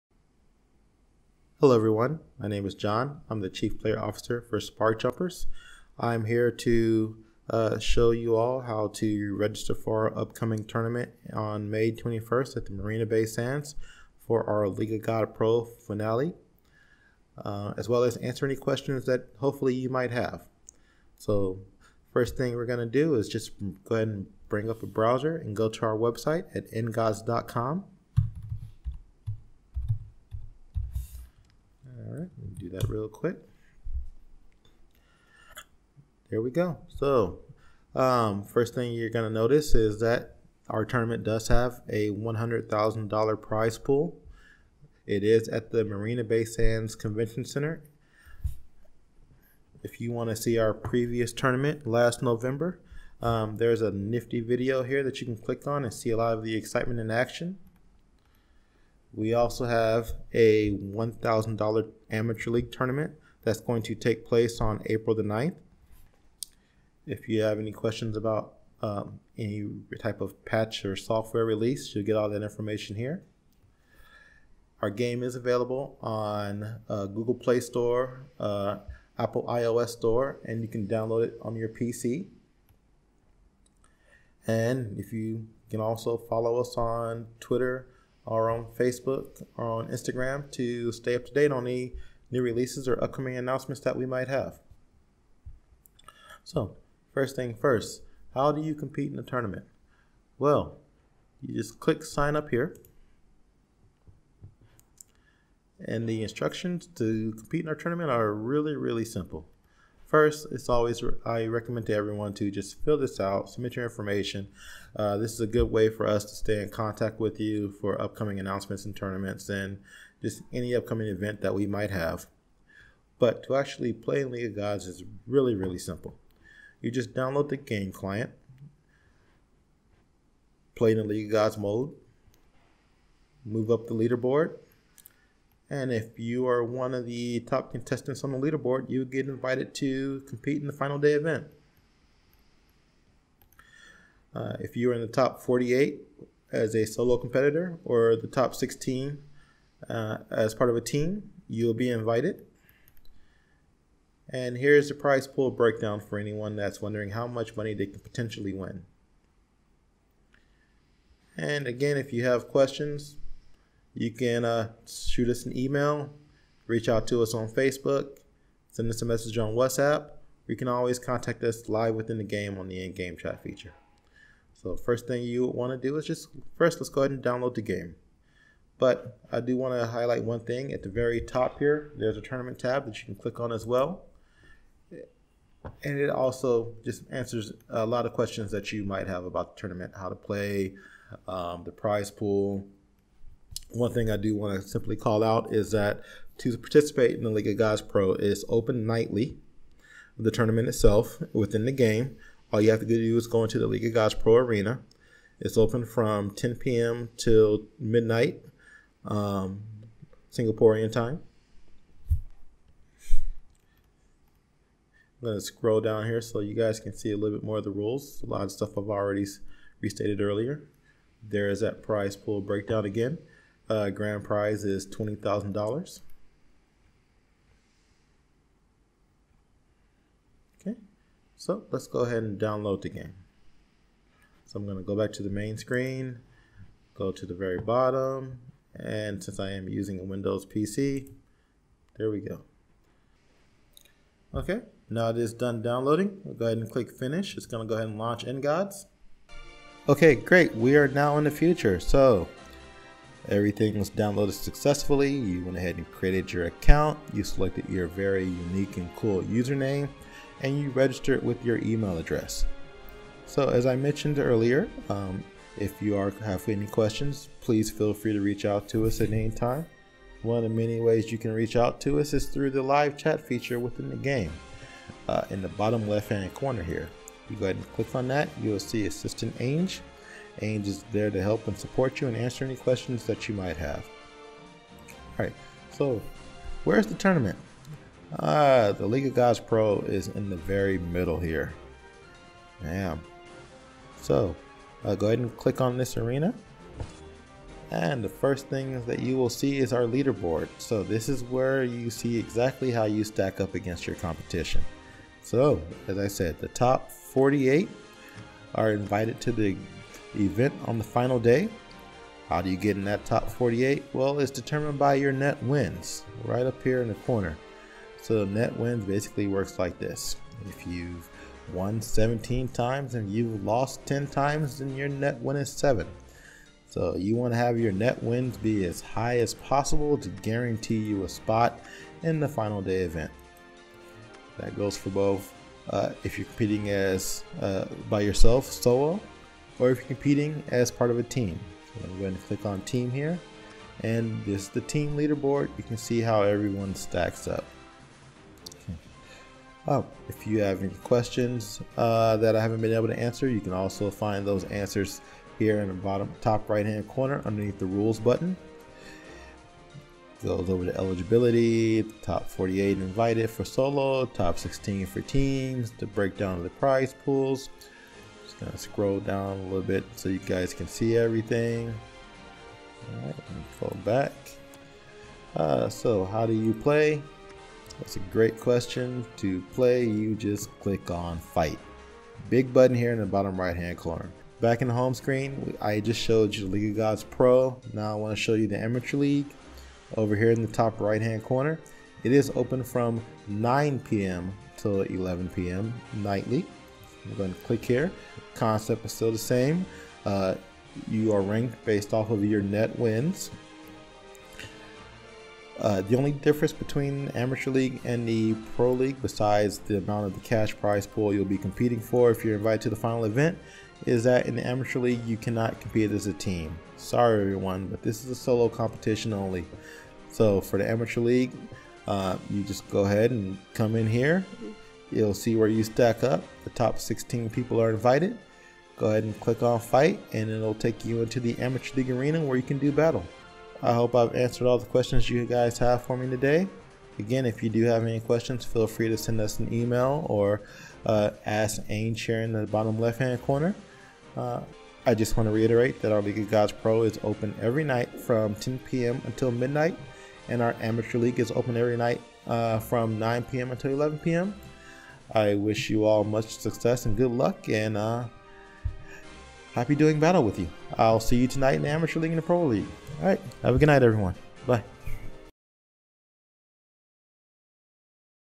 How to Join League of Gods Pro 2016
Get up to speed with this very helpful video tutorial.

Learn how to join in the race for the $100,000 prize pool League of Gods Pro Finals Tournament that is taking place on 21 May 2016 at Marina Bay Sands Singapore

1. EndGods In-Game Live Chat